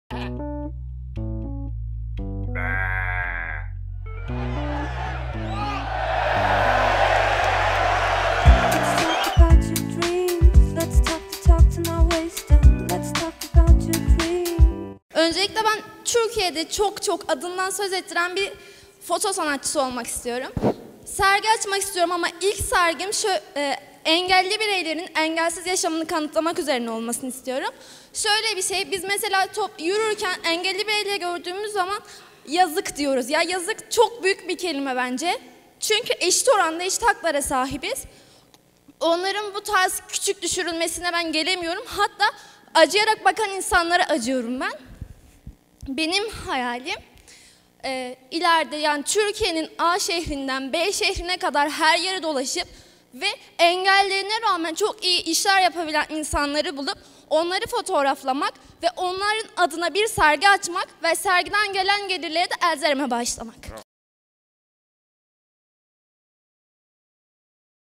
Kamuran Ertürk
''Benim hayalim engelli bireylerin engelsiz yaşamını kanıtlamak üzere bir fotoğraf sergisi açmak.''